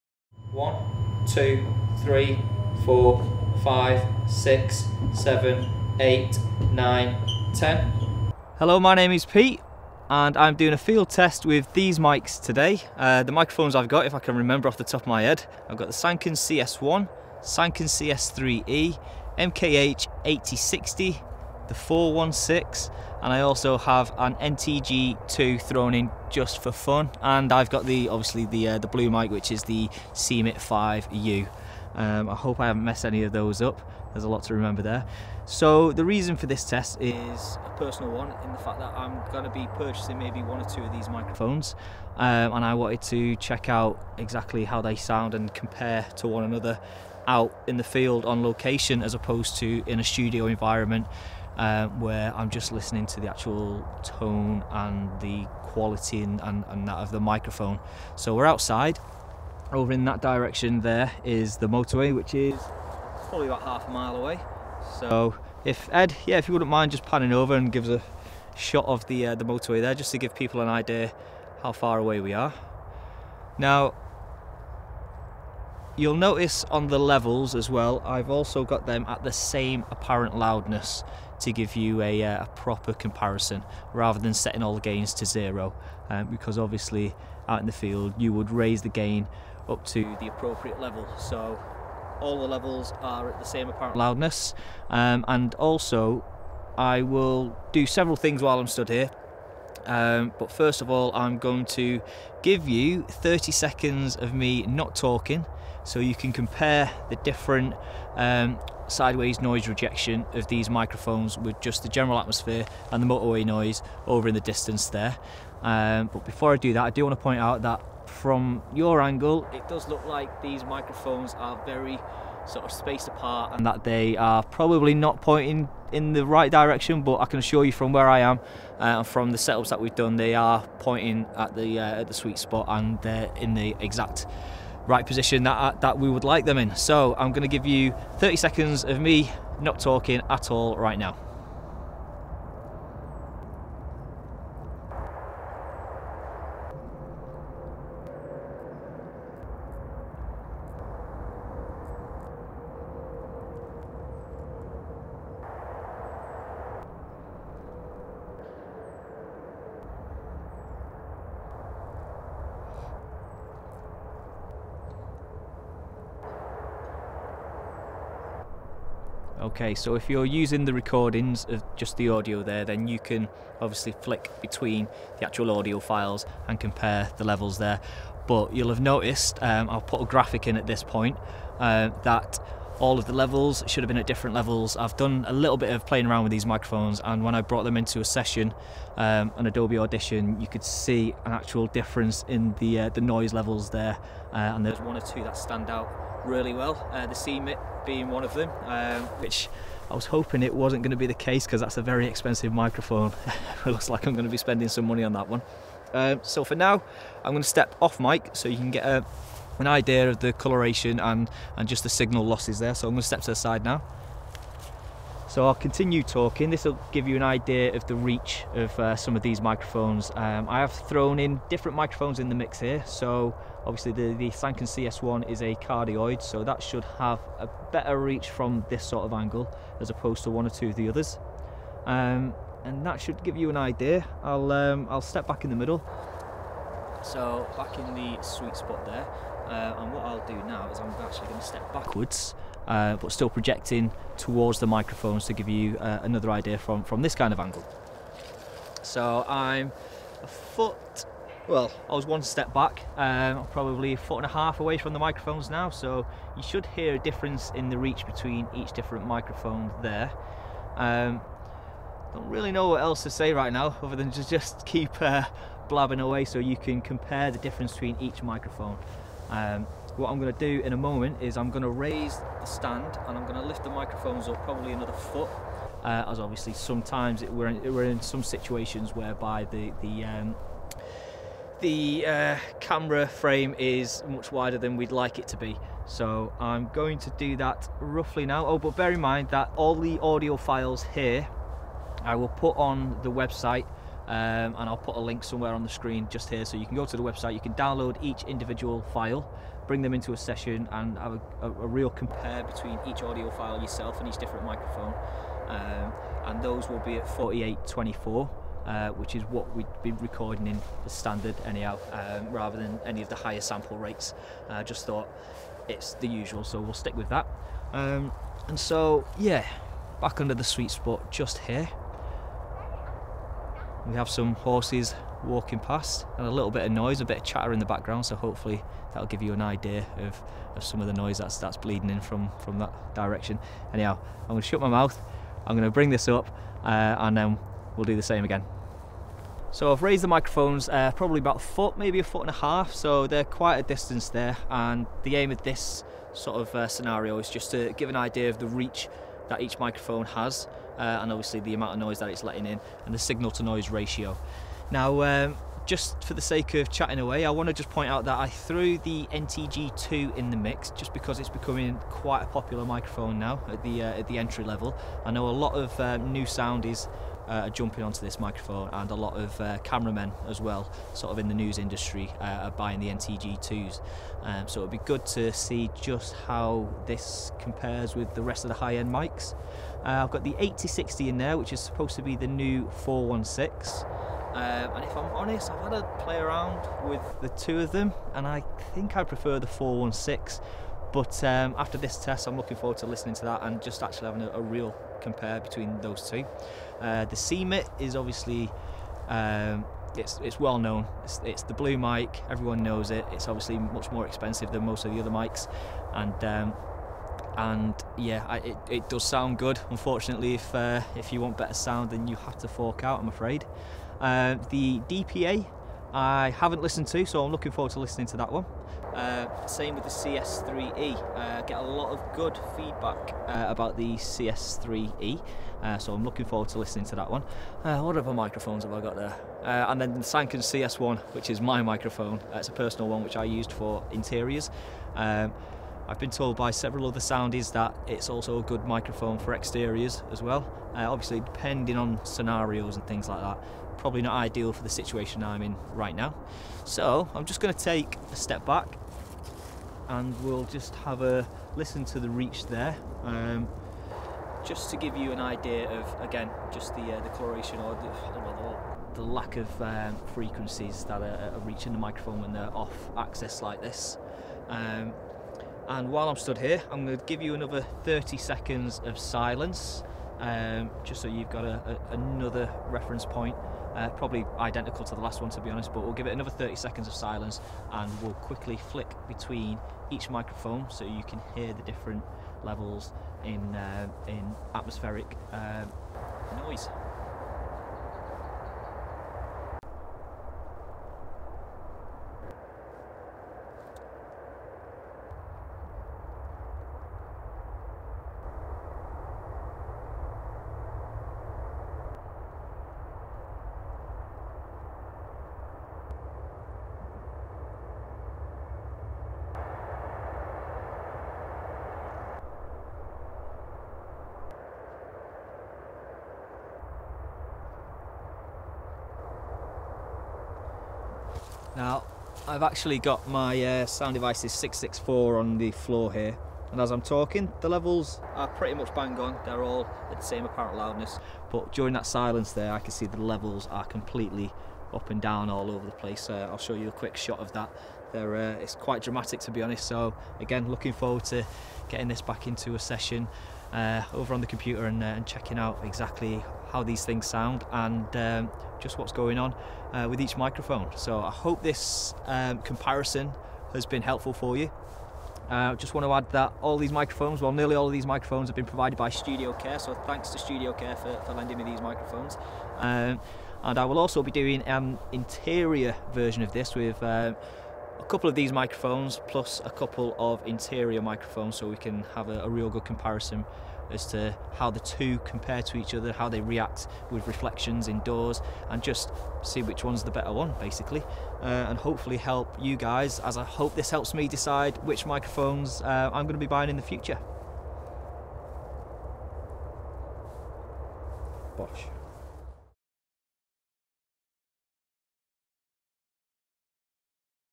1: Mic comparison IN A FIELD using MKH416, MKH8060, CMiT5u, CS3e, CS1, NTG2
This one is out in a field near a motorway...

A comparison (field test) between the Sennheiser mkh 416, mkh8060, Schoeps CMiT5u, Sanken CS3e, Sanken CS1 and Rode NTG2

All mics were recorded on iso tracks so you can download them, loop certain parts, compare and scrutinise them yourself.